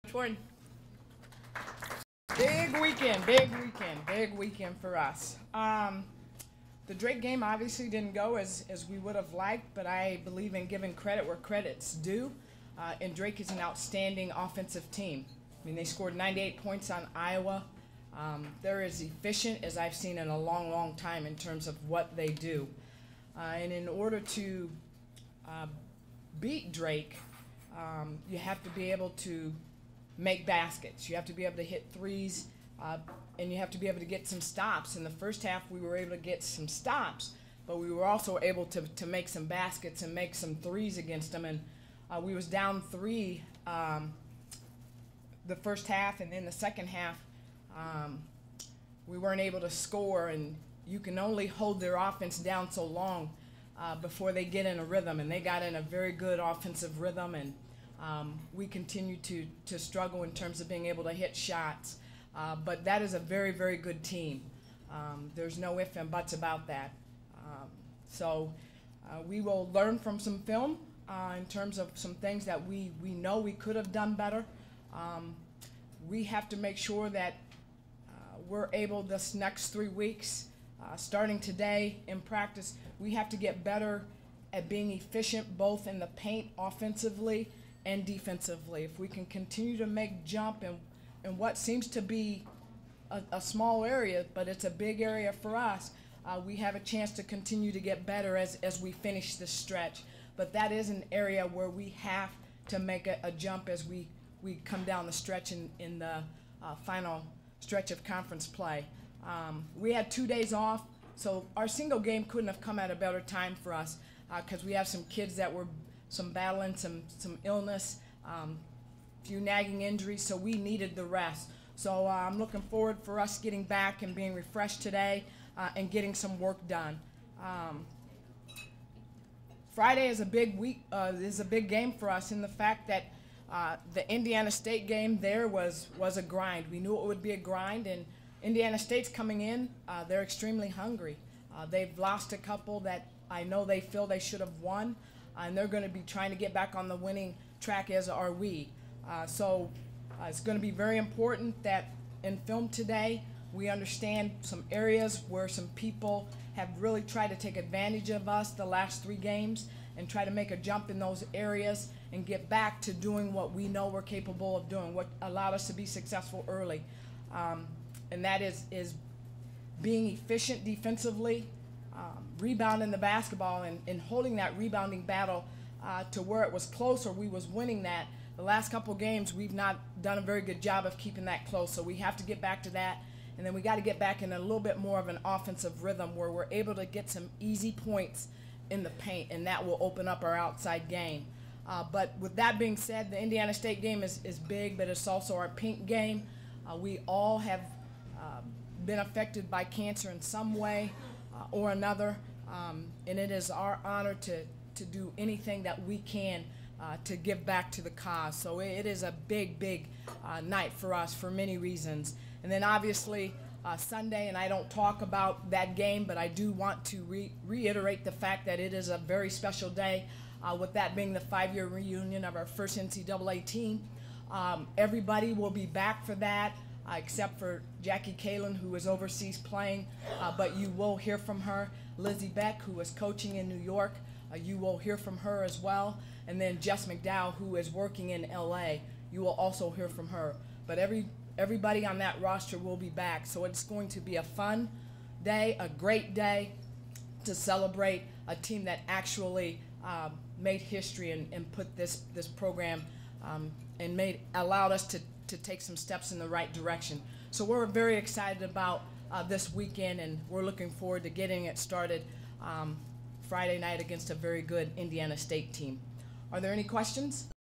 UNI women's basketball press luncheon - Feb. 16, 2015 - recap/look ahead
The UNI women's basketball team hosts two games this week starting 7 p.m. Feb. 20. Fans are encouraged to wear pink to raise awareness of breast cancer. The Panthers return 2 p.m. Feb. 22 and will sign autographs at the end of the game. The first The team will be honoring the first UNI women's basketball team to make the NCAA tournament. Members of that team will be on hand.